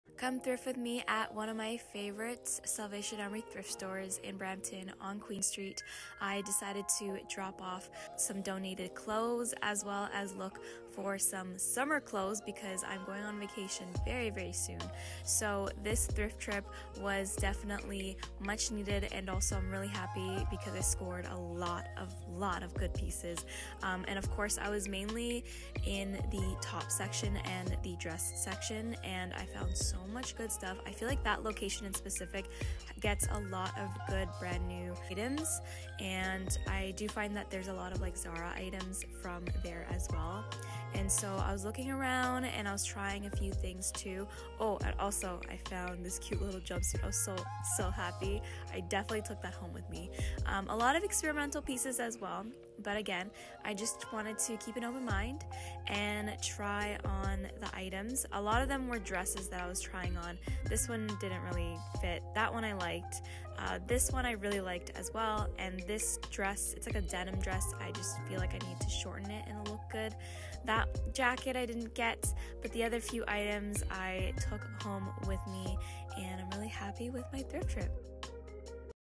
Clutter-Free February: From Decluttering to Vacation
Turning closet clutter into vacation-wear! ♻️ Anna dropped off clothing she no longer needed to her local Brampton South Donor Welcome Centre for ClutterFreeFebruary and received a 20% off Thank You coupon to prep for her summer by the sea 🏖️ Have you participated in Clutter-Free February yet?